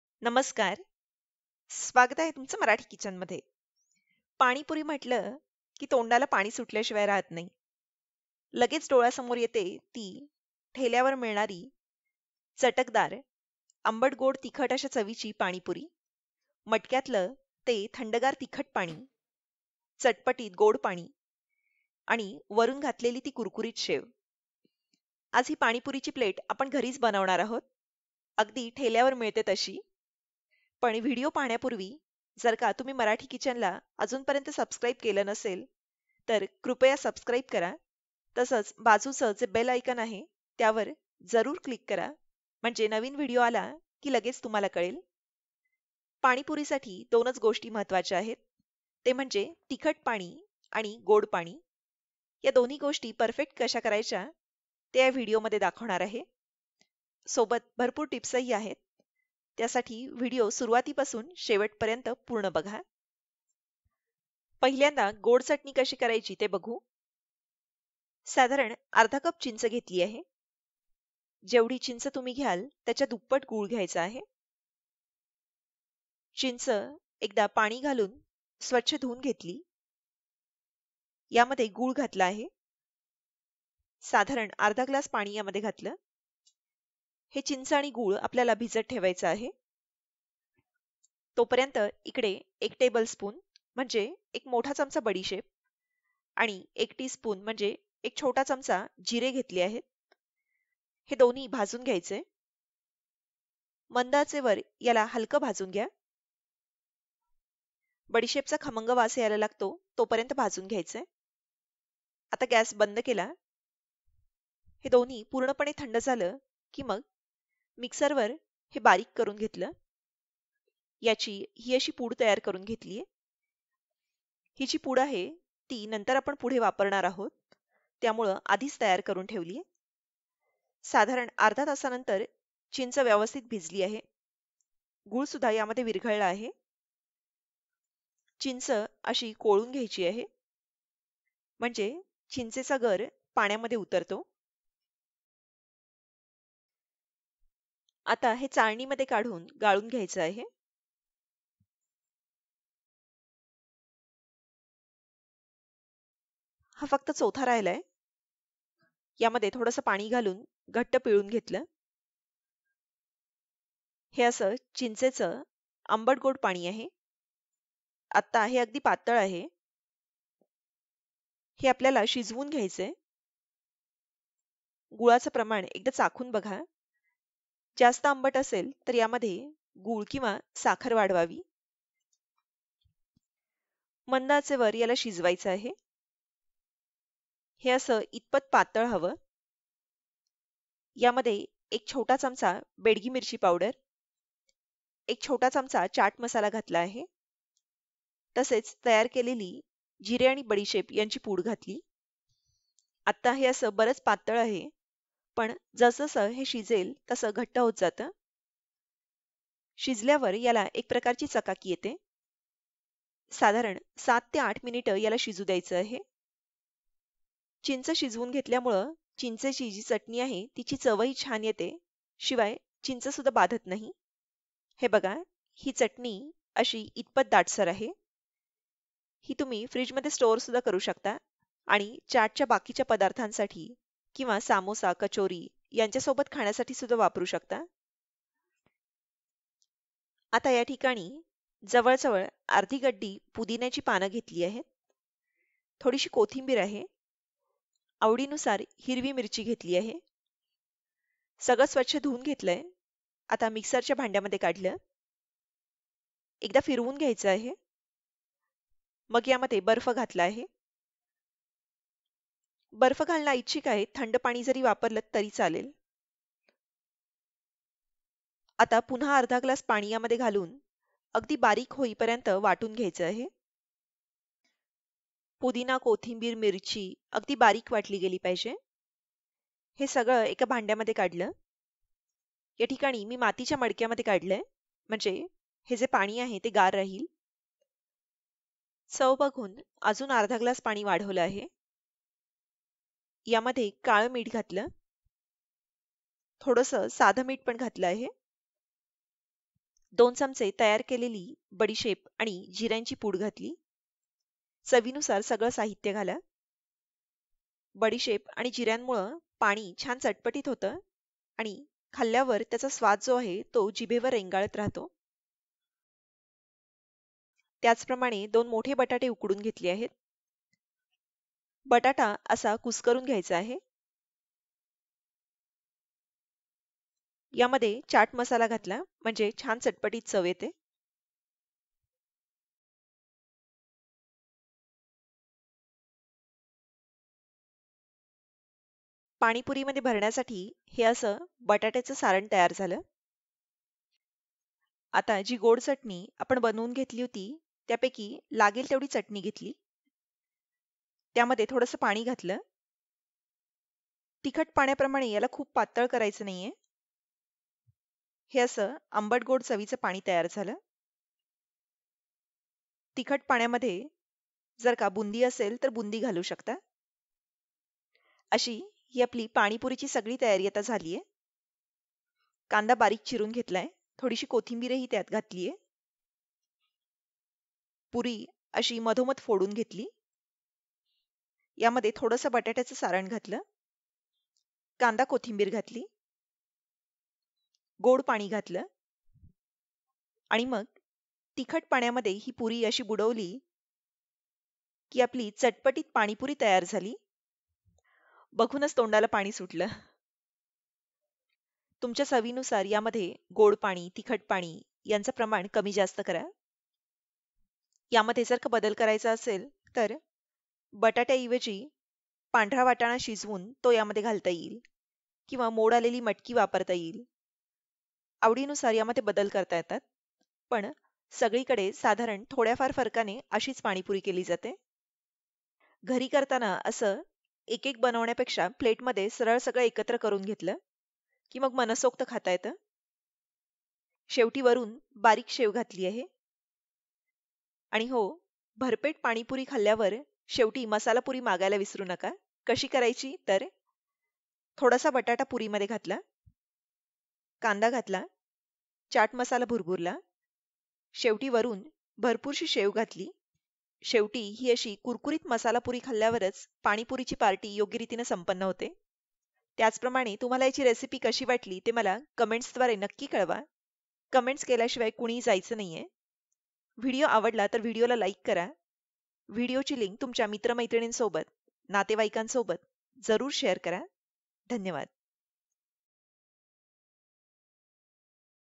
ठेल्यावर मिळते तशी मटक्यातली थंडगार चटपटीत पाणीपुरी | गोड-तिखट पाणी बनवायची परफेक्ट पद्धत | Panipuri
Dosa Tawa
Mixing bowl
Grill Toaster
Non-stick fry pan
Appe pan